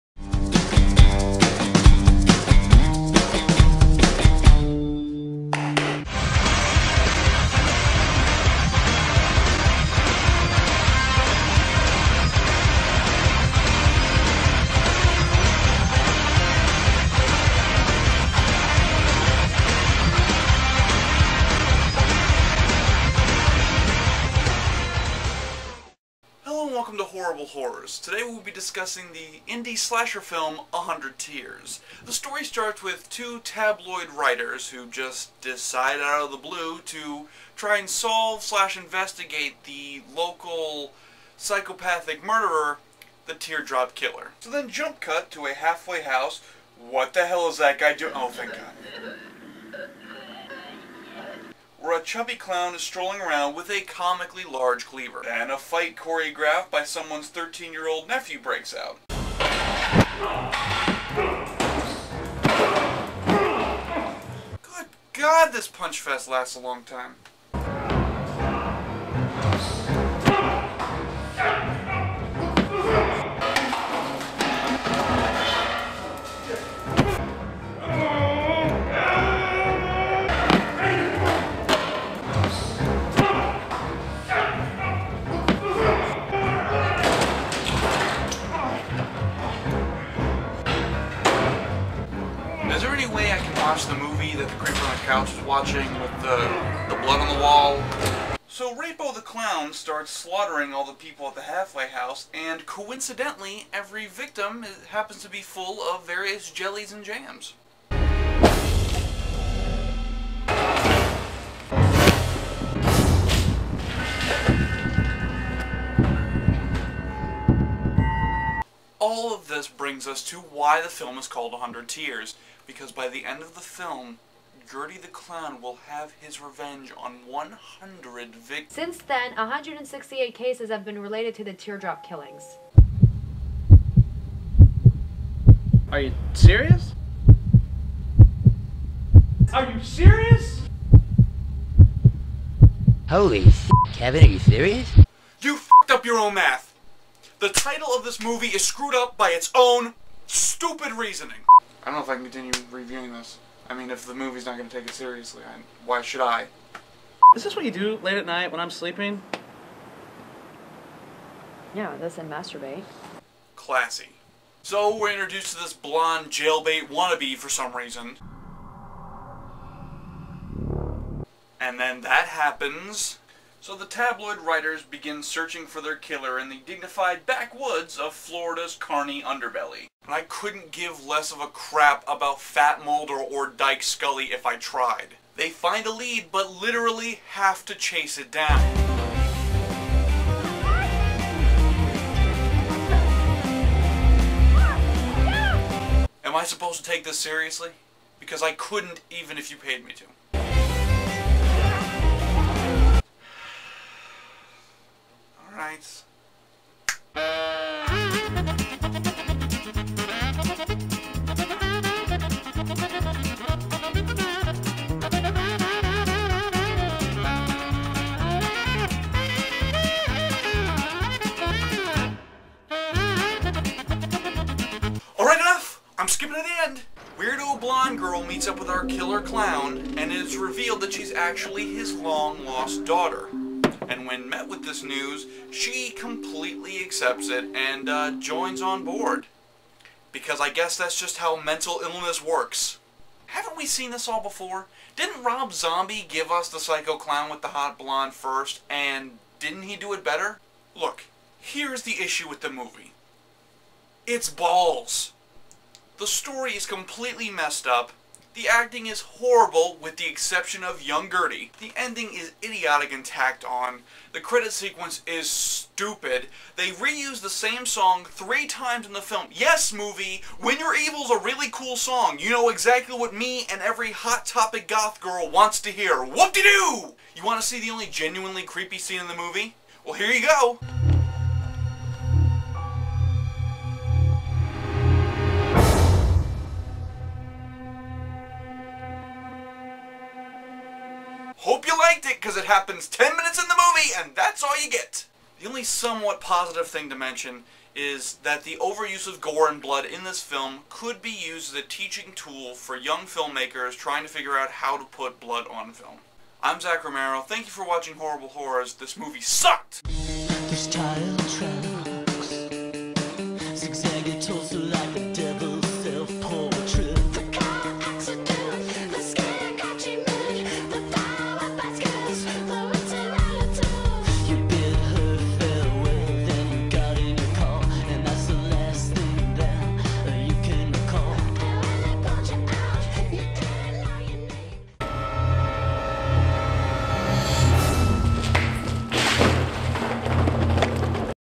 100 Tears [Horrible Horrors]
Zac tackles the independent clown killer thriller